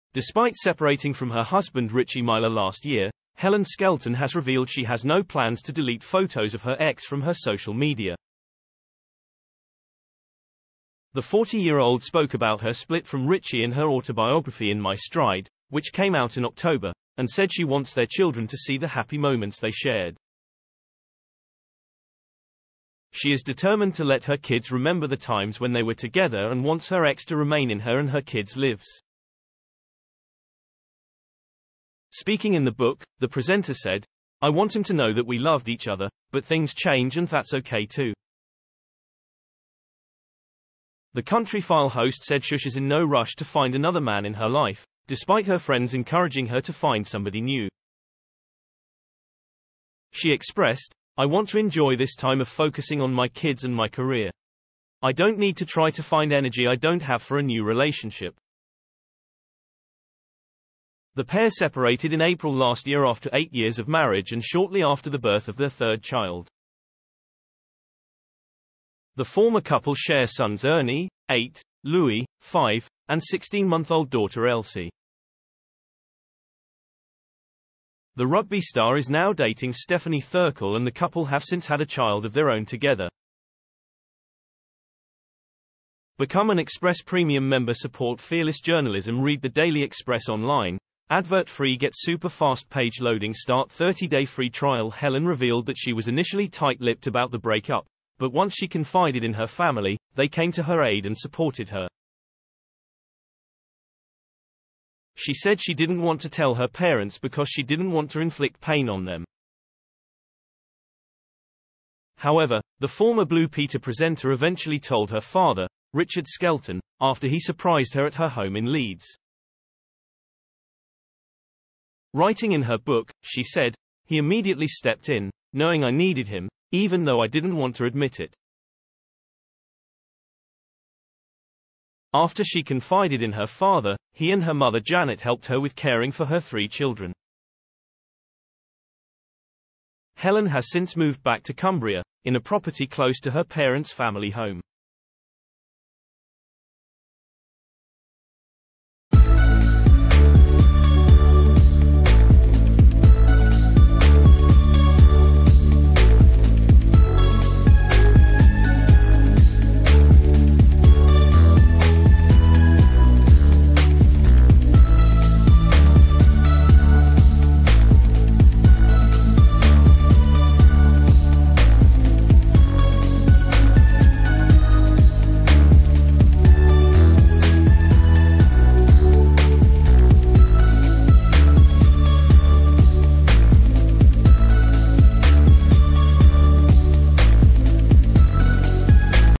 Helen Skelton shares heartbreaking reason she won't delete photos of ex Richie
Despite separating from her husband Richie Myler last year, Helen Skelton has revealed she has no plans to delete photos of her ex from her social media. The 40-year-old spoke about her split from Richie in her autobiography In My Stride, which came out in October, and said she wants their children to see the happy moments they shared. She is determined to let her kids remember the times when they were together and wants her ex to remain in her and her kids' lives. Speaking in the book, the pre...